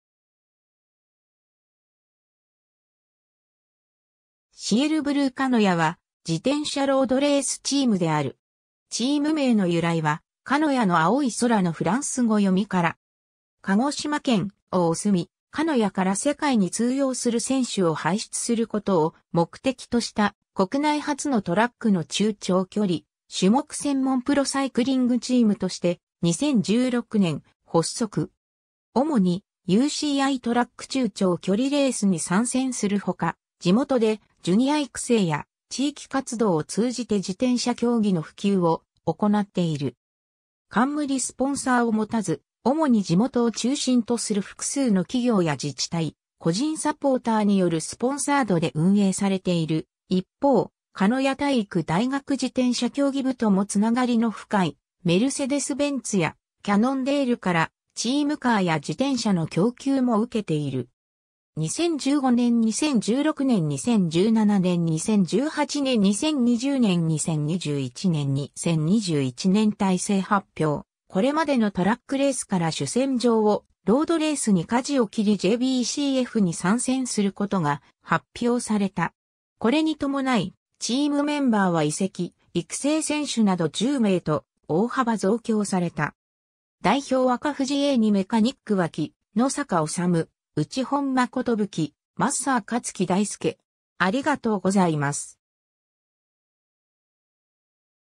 シエルブルー鹿屋
シエルブルー鹿屋（Ciel Bleu KANOYA）は、自転車ロードレースチームである。
 チーム名の由来は「鹿屋の青い空」のフランス語読み(la Ciel Bleu KANOYA)から。
 鹿児島県「おおすみ・鹿屋」から世界に通用する選手を輩出することを目的とした国内初のトラックの中長距離種目専門プロサイクリングチームとして、2016年発足。
 主にUCIトラック中長距離レースに参戦するほか、地元でジュニア育成や地域活動を通じて自転車競技の普及を行っている。
 冠スポンサーを持たず、主に地元を中心とする複数の企業や自治体(鹿屋市)、個人サポーターによるスポンサードで運営されている一方、鹿屋体育大学自転車競技部ともつながりの深いメルセデス・ベンツやキャノンデールからチームカーや自転車の供給も受けている。
 2015年 2016年 2017年 2018年 2020年 2021年 2021年体制発表、これまでのトラック・レースから主戦場をロードレースに舵を切りJBCF(J Proツアー)に参戦することが発表された。
 これに伴い、チームメンバーは移籍・育成選手など10名と大幅増強された。
 代表 若藤英二 メカニック　脇野栄治・家本真寿 マッサー　香月大輔